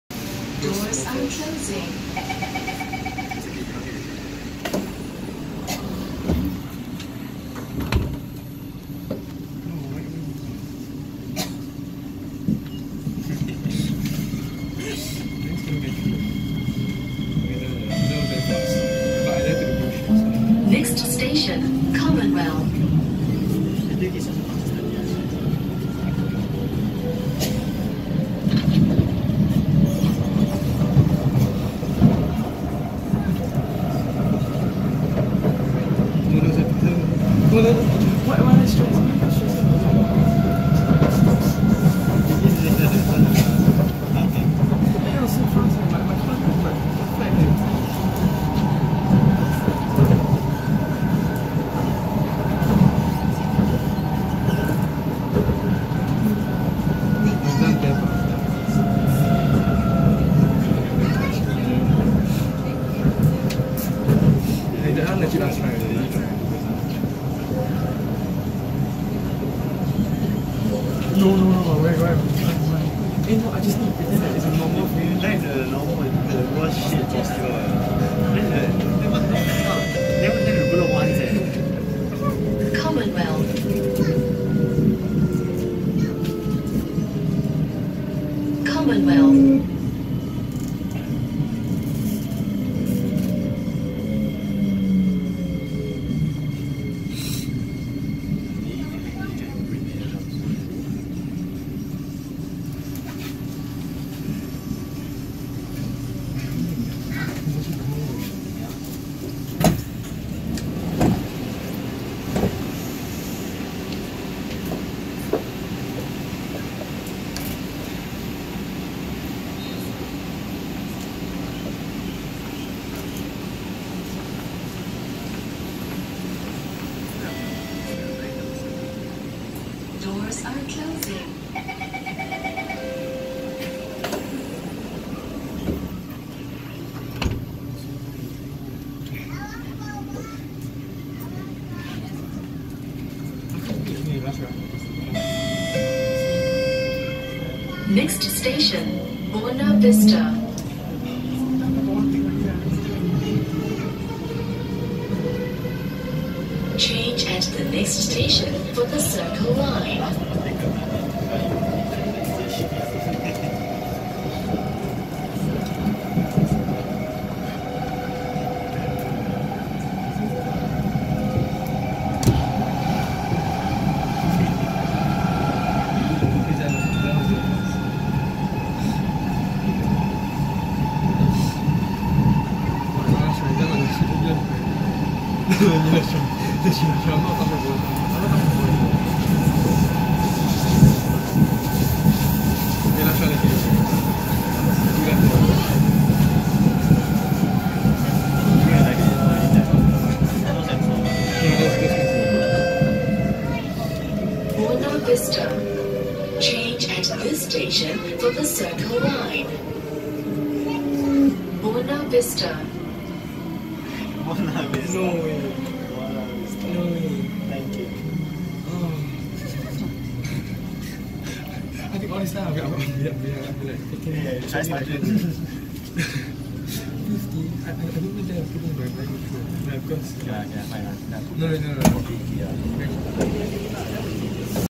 [Strong deceleration] C751B 311/312 [EW21 Queenstown to EW19 Buona Vista]
C751B 311/312 makes a strong deceleration sound, which is surprising considering how long the C751B rolling stock has been in service. Still my favorite rolling stock by a long shot!

Another thing is, I'm quite surprised at how much Queenstown and Commonwealth has changed over the years now it's all high rise HDBs and Condos when 10 years ago it was all grass fields. C751B 311/312 has a strong deceleration motor sound, which is surprising considering that the C751B rolling stock has been in service for more than 15 years already.

Journey rating: 8.5/10